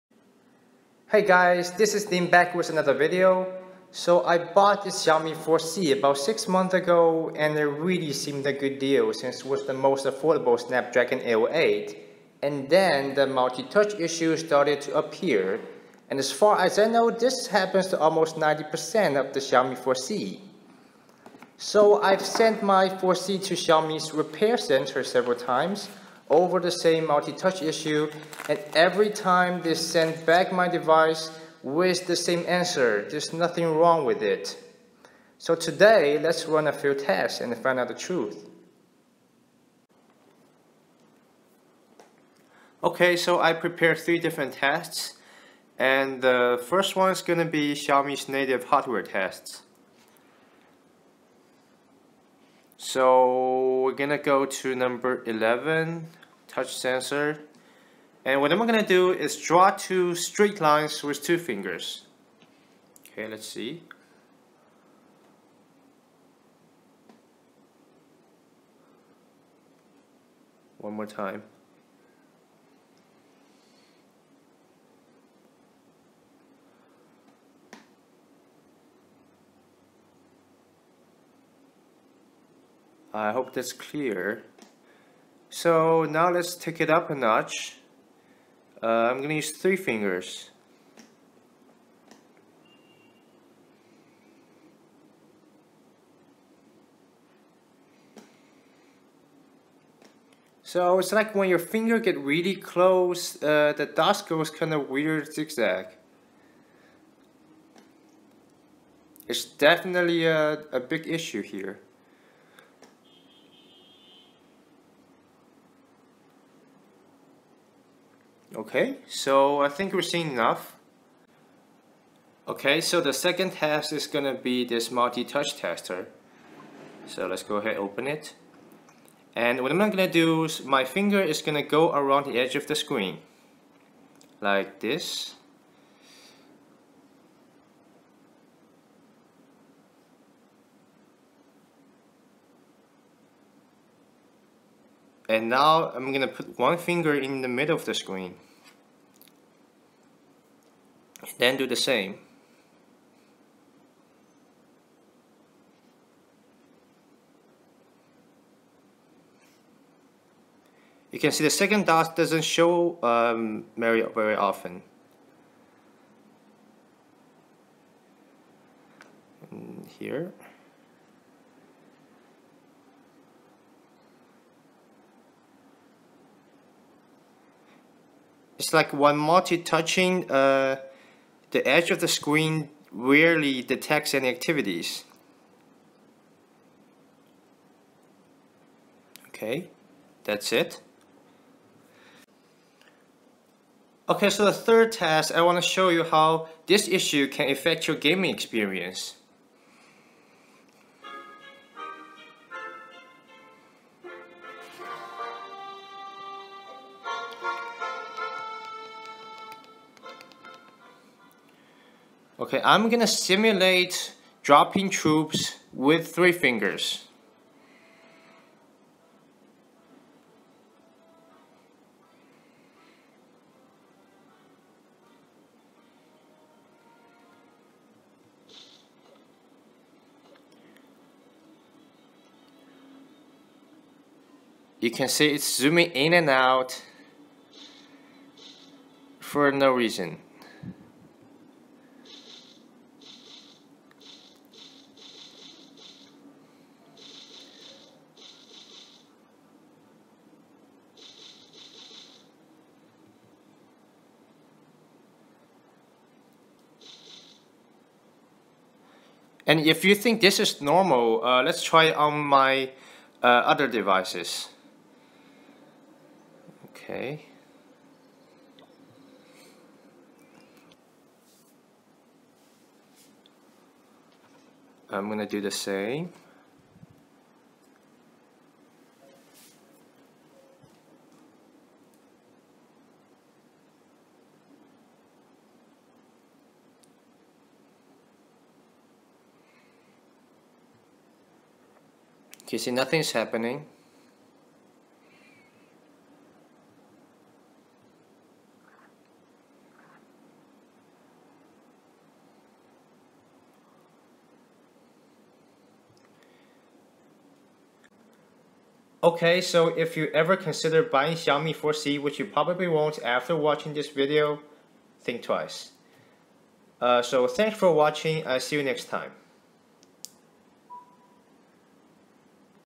[DO NOT BUY] irritating multitouch issue on Xiaomi 4C
irritating multitouch issue on Xiaomi 4C
All the tests are based MIUI 7.3.1.0 stable version. I also tried flashing sMIUI, Cyanogenmod 12.1&13, but the problem remains.